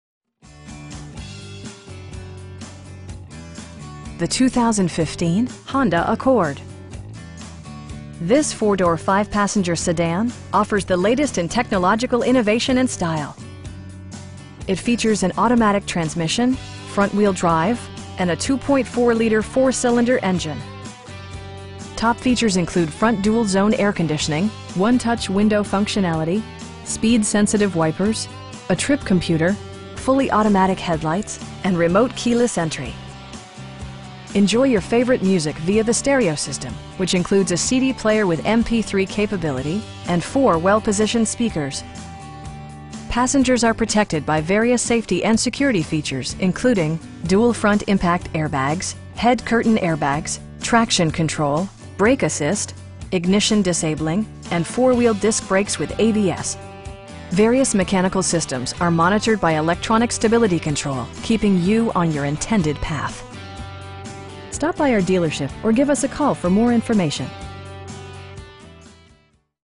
2015 Honda Accord LX in St Augustine, FL 32086
The 2015 Honda Accord. This 4 door, 5 passenger sedan offers the latest in technological innovation and style. It features an automatic transmission, front-wheel drive, and a 2.4 liter 4 cylinder engine. Top features include front dual zone air conditioning, 1-touch window functionality, speed sensitive wipers, a trip computer, fully automatic headlights, tilt and telescoping steering wheel, an overhead console, and remote keyless entry. Passengers are protected by various safety and security features, including: dual front impact airbags, head curtain airbags, traction control, brake assist, ignition disabling, and 4 wheel disc brakes with ABS. Various mechanical systems are monitored by electronic stability control, keeping you on your intended path. Our aim is to provide our customers with the best prices and service at all times. Stop by our dealership or give us a call for more information.